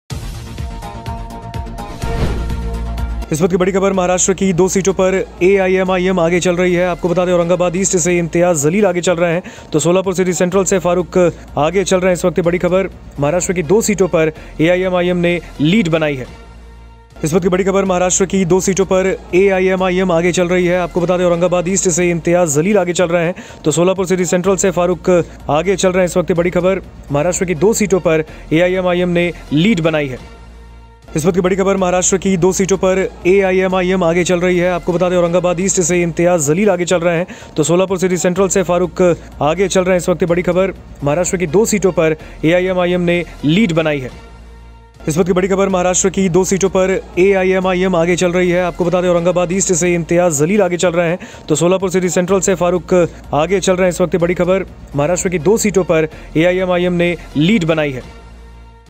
Maharashtra Result: AIMIM 2 सीटों पर बना रही बढ़त, अभी कुछ राउंड की गिनती बाकी
Maharashtra की 288 विधानसभा सीटों के लिए एक चरण में वोटिंग हुई थी, जबकि Jharkhand की 81 सीटों पर दो चरणों में मतदान हुआ था.  क्या Maharashtra में Eknath Shinde और Jharkhand में Hemant Soren सत्ता में वापसी कर पाते हैं या दोनों ही राज्यों को कोई नया मुख्यमंत्री मिलेगा ?
देखिए Maharashtra और Jharkhand विधानसभा चुनाव के नतीजे सबसे तेज़ सिर्फ News24 पर .
साथ ही UP By election 2024 के नतीजे भी घोषित होंगे ,यहाँ मुख्य मुकाबला BJP vs Samajwadi Party के बीच है |